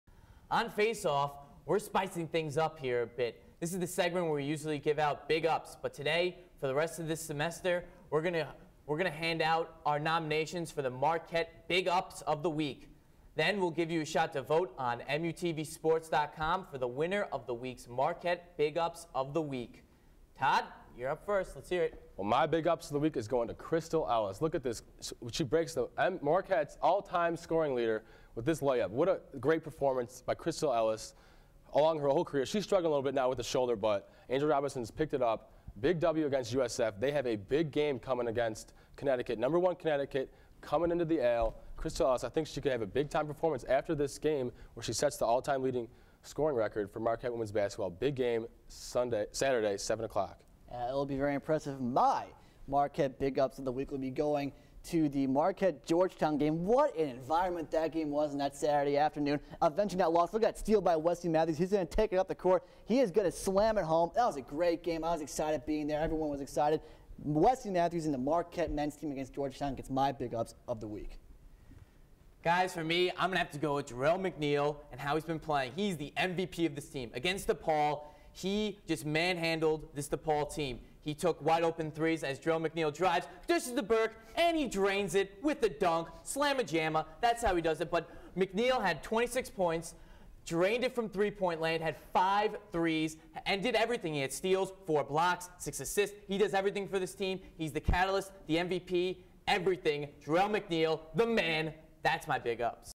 MUTV "Marquette Big Ups of the Week: Jan. 30-Feb. 5"
MUTV's "FaceOff" guys, Brian Hoffman, Donnie Dwyer, and Todd Warner, hand out their nominations for the "Marquette Big Ups of the Week" for the time between Friday, Jan. 30 and Thursday, Feb. 5, 2009.
Check out MUTVsports.com to vote for the weekly winner!